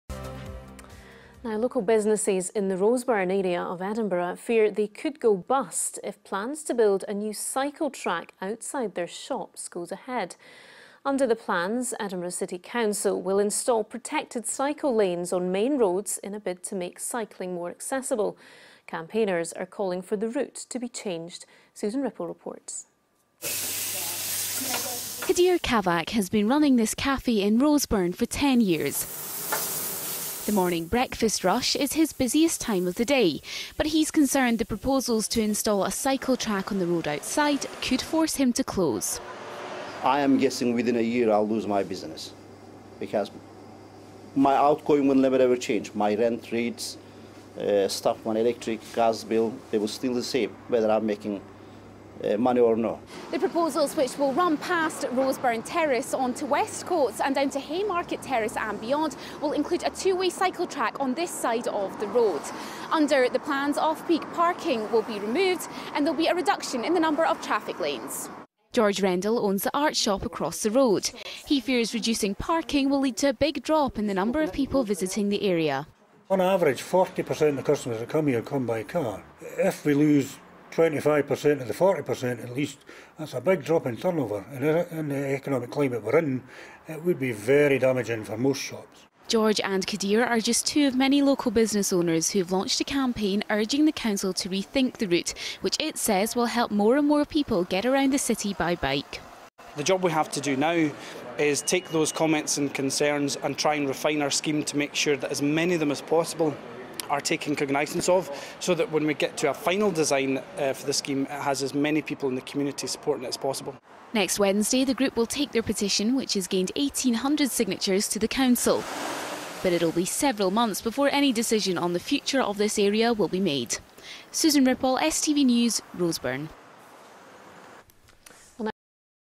STV- West Coates Cycle Track Protest
STV Edinburgh Broadcast 28.01.16 on Traders protest again West Coates Cycle Track
This videos was to promote the campaign that KnS was running against the CCWEL cycle link.